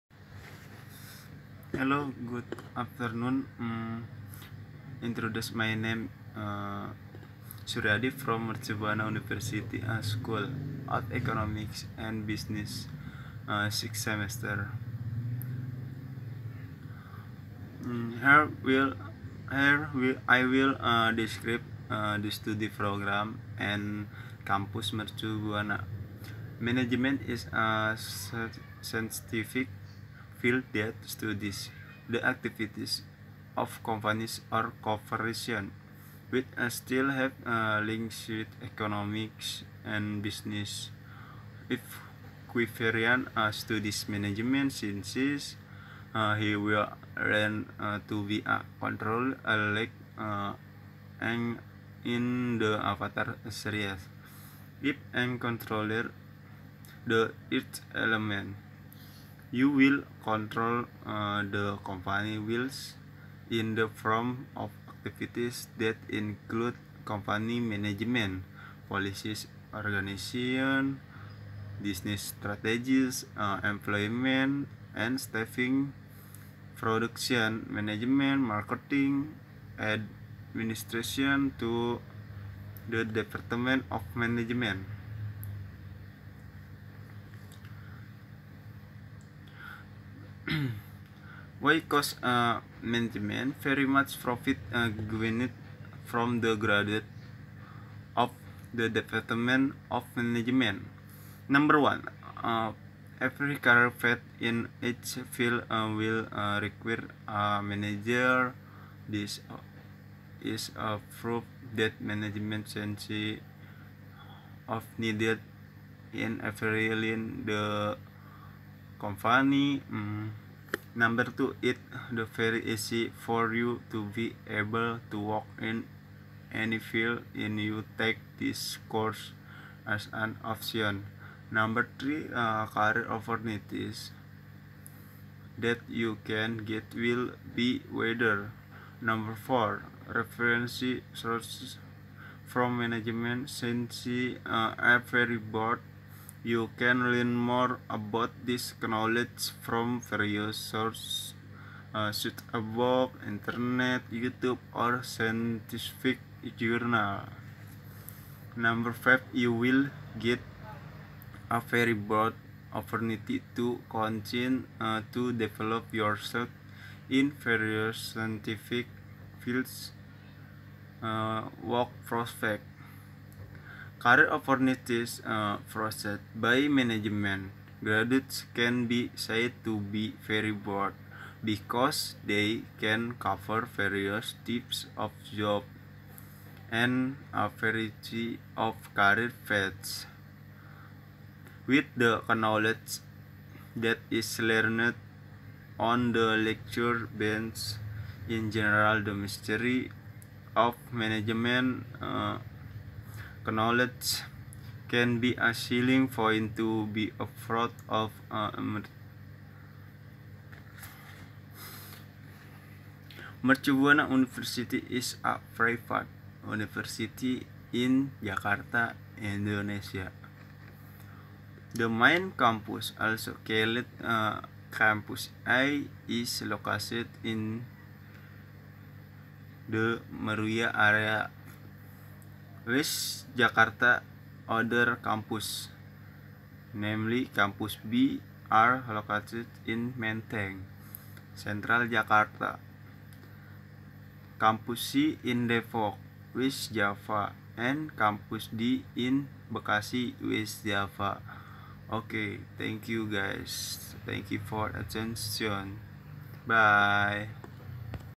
Suryadi Tugas Besar 1 : Describing prodi and campus.
Hello good afternoon, introduce my name Suryadi from Mercu Buana University School of Economics and Business 6th semester, here I will describe the study program, and campus mercubuana.
Management is a scientific field that studies the activities of companies or corporations, which still have links with economics and business. If Quipperian studies Management Science, he will learn to be a "controller" like Aang in the Avatar series. If Aang controls the earth element, you will control the company's wheels in the form of activities that include company management policies, organizations, business strategies, employment and staffing, production management, marketing, administration, to nonprofit organizations.
Why Choose Management?
Very much profit gained from a graduate of the Department of Management.
1. Every career path in each field will require a manager. This is proof that Management Science is needed in every line in the company.
2. It is very easy for you to be able to work in any field if you take this course as an option.
3. Career opportunities that you can get will be wider.
4. Reference sources from Management Science are very broad. You can learn more about this knowledge from various sources, such as books, internet, YouTube, or scientific journals.
5. You will get a very broad opportunity to continue to develop yourself in various scientific fields.
Work prospect
Career opportunities possessed by management graduates can be said to be very broad because they can cover various types of jobs and a variety of career paths. With the knowledge that is learned on the lecture bench, in general the mastery of management knowledge can be a selling point to be proud of.
Mercu Buana University is a private university in Jakarta, Indonesia.